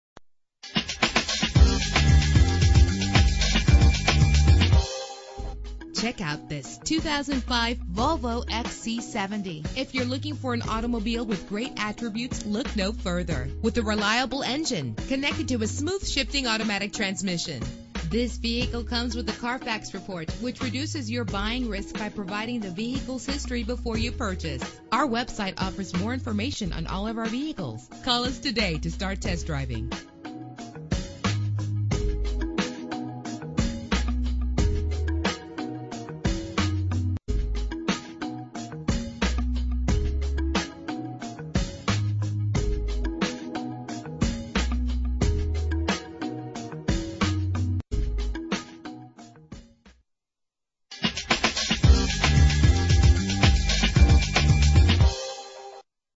2005 Volvo XC70 - Manheim PA
Year: 2005
Make: Volvo
Model: XC70
Trim: 2.5T Wagon 4D
Engine: 5 cylinder LP Turbo 2.5 liter
Transmission:
Color: *Not Listed
Mileage: 112044
Address:
14 Anthony Drive